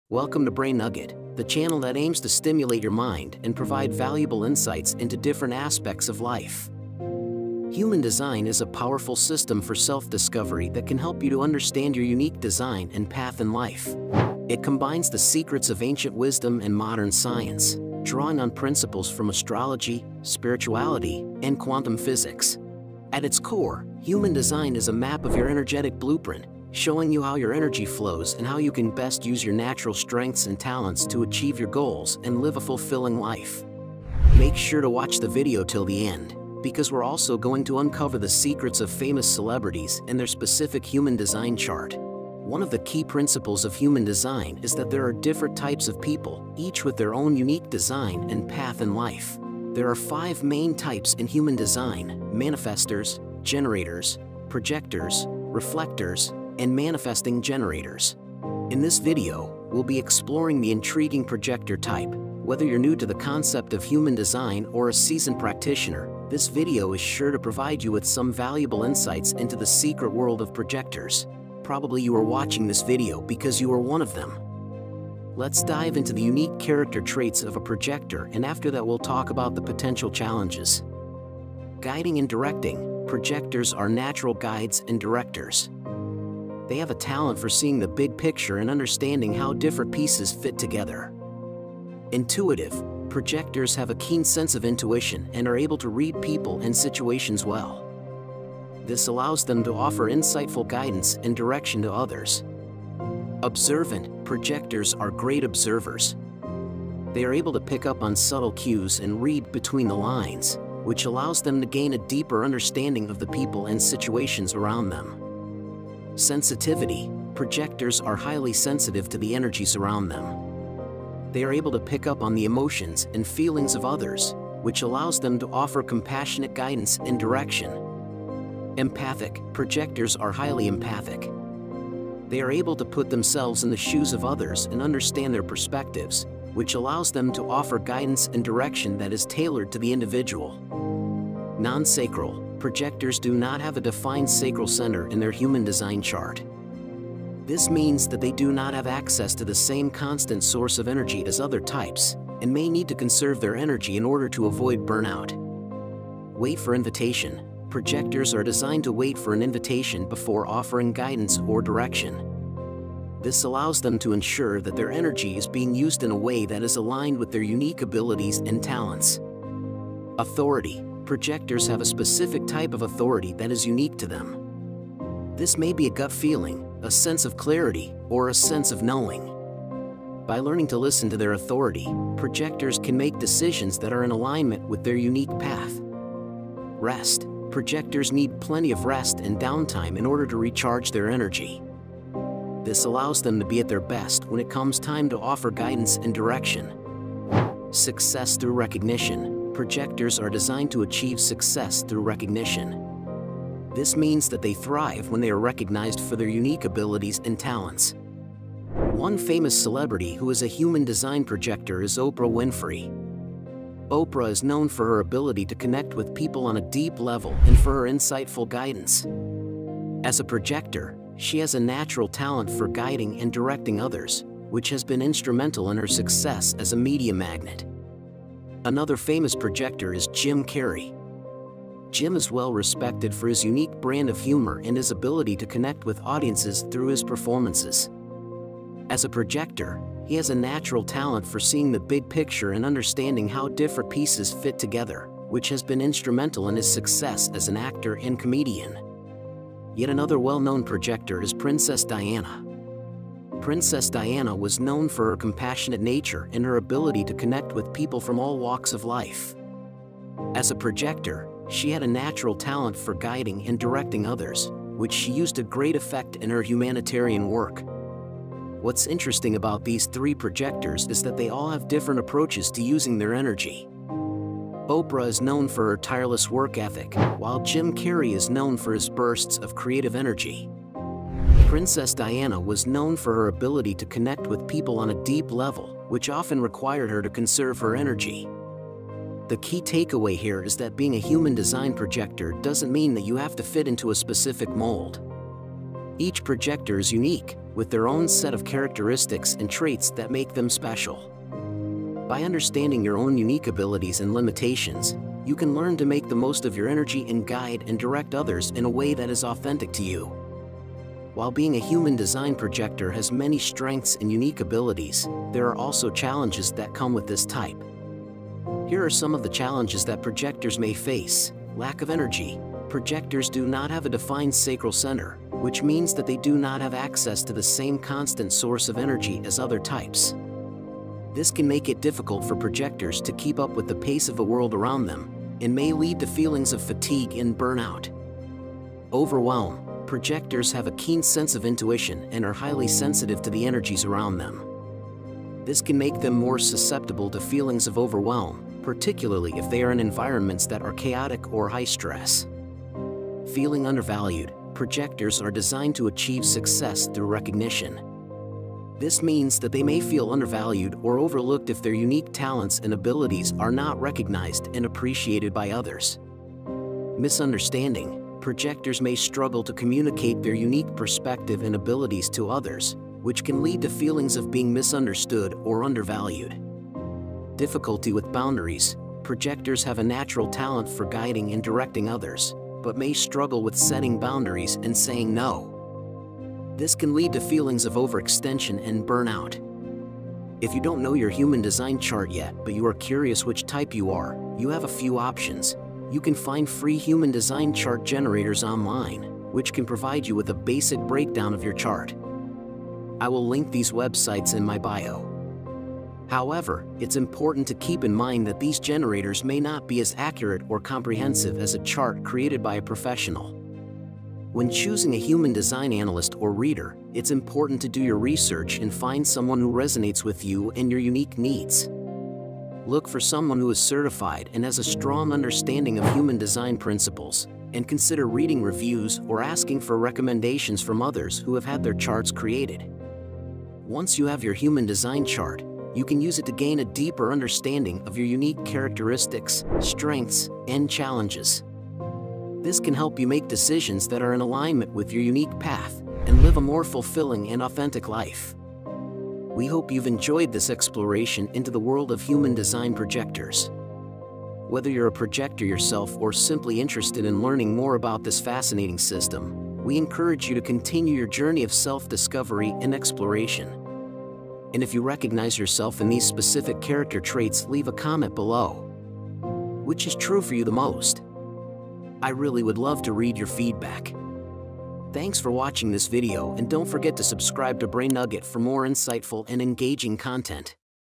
The Secret Character Traits of a Human Design PROJECTOR
In this eye-opening video, we delve into the enigmatic world of Human Design Projectors and unveil their secret character traits. As unique and rare individuals, Projectors possess distinct qualities that shape their experiences and interactions with the world. Join our expert insights as we reveal the hidden facets of Projector energy, including their inherent wisdom, sensitivity, intuition, and more.

Discover how understanding these secret character traits can empower you to live authentically and thrive as a Projector in your Human Design. Don't miss this exclusive opportunity to unravel the mysteries of Projector traits and unlock your true potential!

I will try my best to reply to all the comments!